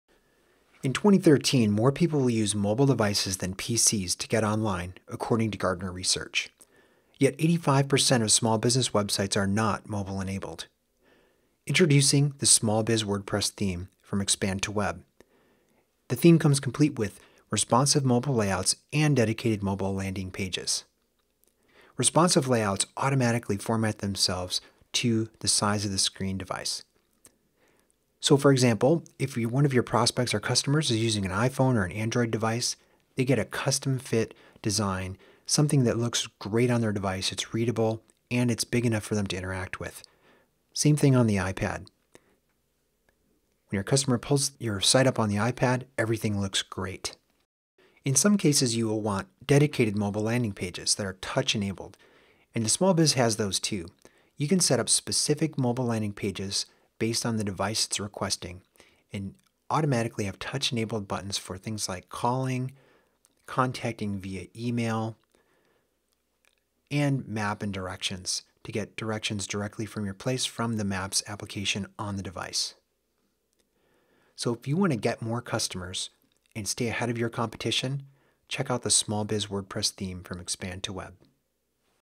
SmallBiz Dynamic WordPress Theme - Mobile Responsive Layout
This video shows the responsive layout for the new SmallBiz Dynamic WordPress Theme. This allows your website to automatically show up sized perfectly for whatever mobile device your visitors are using, including iPhone, iPad, Android, or any mobile device.